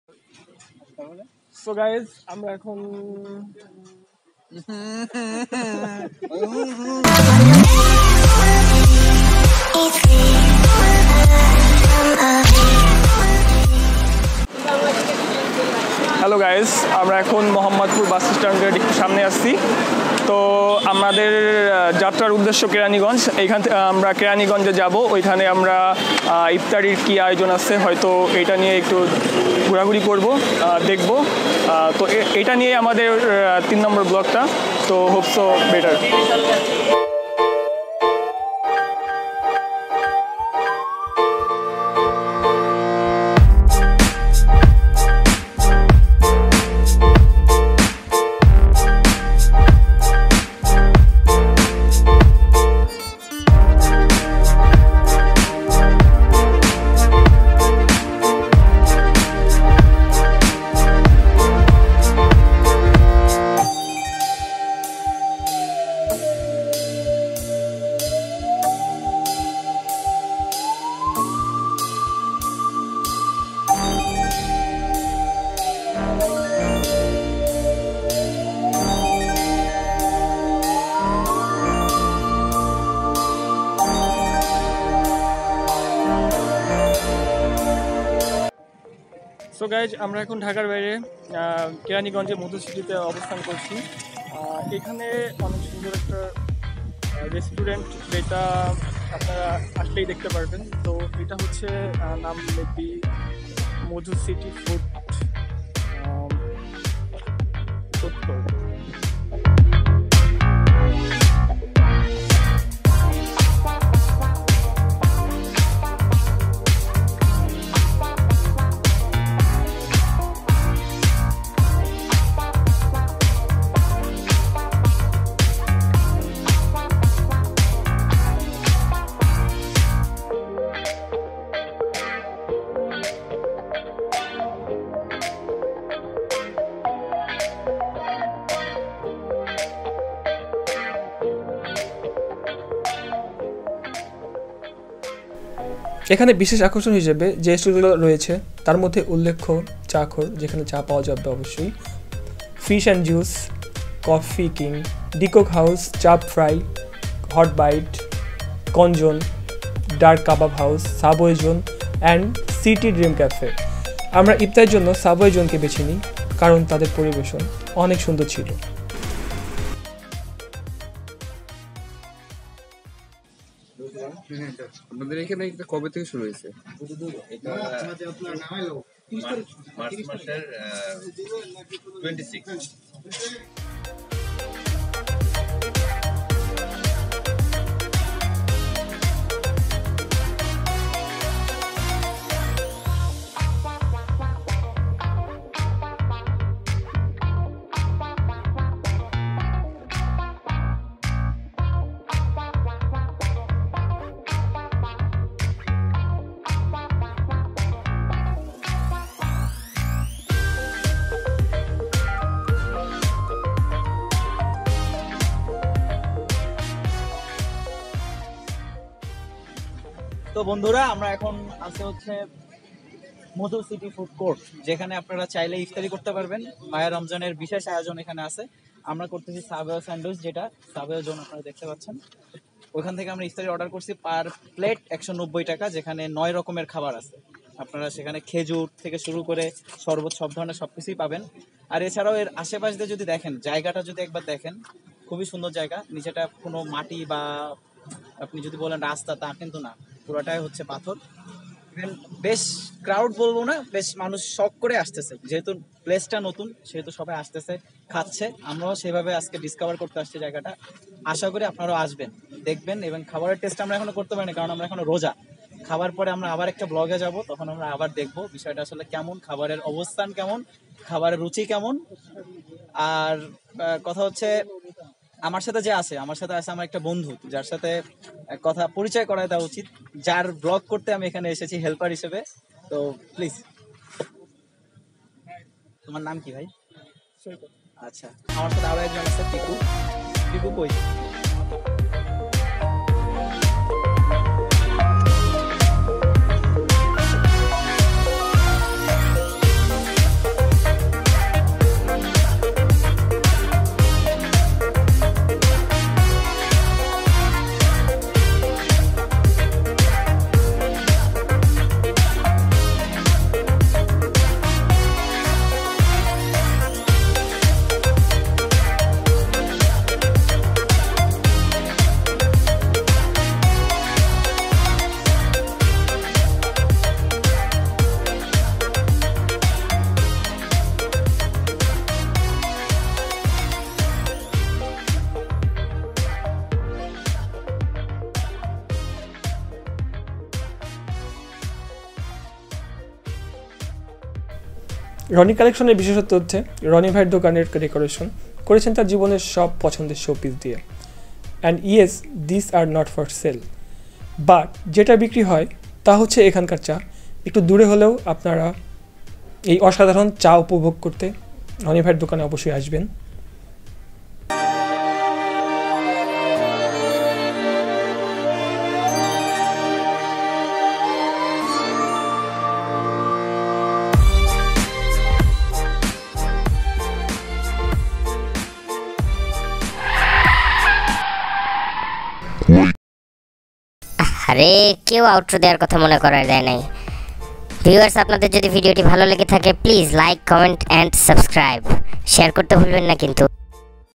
Modhu City Food Court | Rony Collection | Keraniganj || VLOG 3
A Casual trip to a new place @ new Madhu City, Keraniganj. it was interresting trip for us because it was a new place not too crowded minimum distance.

watch it full too lern more about this place specially Rony collecction.

Keep smiling and support us